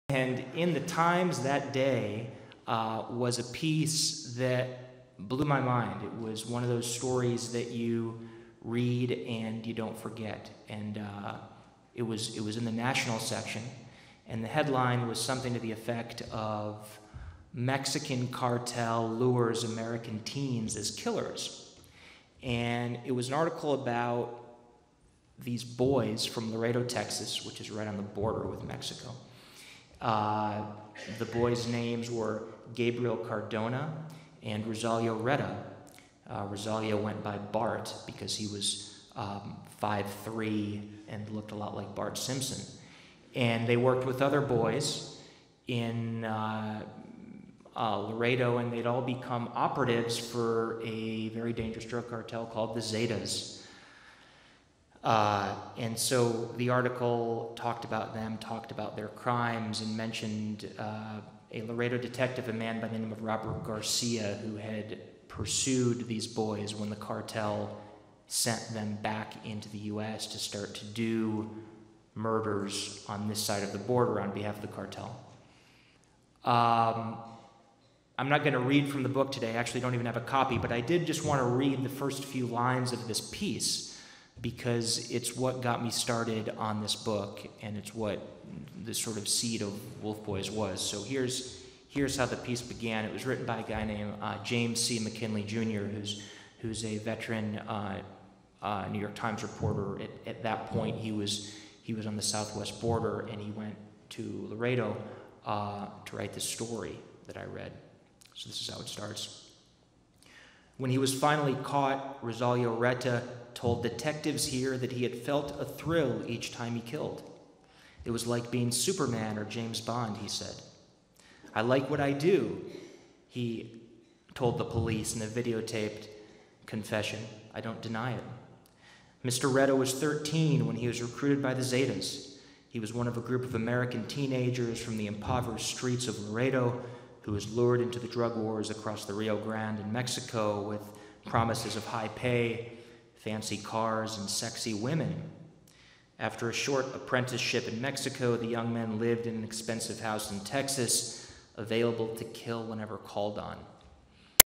2017 Savannah Book Festival: Dan Slater, "Wolf Boys"
Dan Slater examines both sides of the "war on drugs" in his book, "Wolf Boys" which follows two Laredo, Texas teenagers involvement with a Mexican drug cartel and a Mexican born detective tracking the boys and the Cartel.